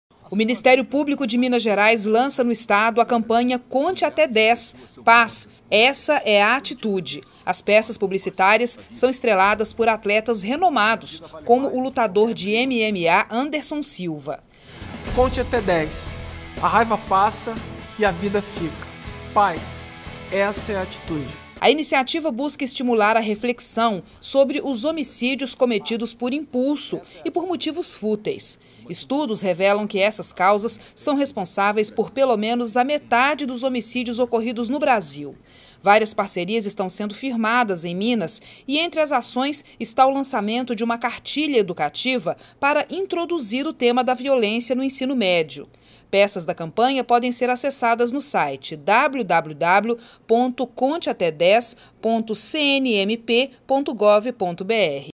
Ministério Público lança em Minas campanha contra violência
O Ministério Público de Minas Gerais lança no Estado a campanha "Conte até 10. Paz. Essa é a atitude". As peças publicitárias são estreladas por atletas renomados e buscam estimular a reflexão sobre os homicídios cometidos por impulso e por motivos fúteis. Várias parcerias estão sendo firmadas em Minas e entre as ações está o lançamento de uma cartilha educativa para introduzir o tema da violência no Ensino Médio.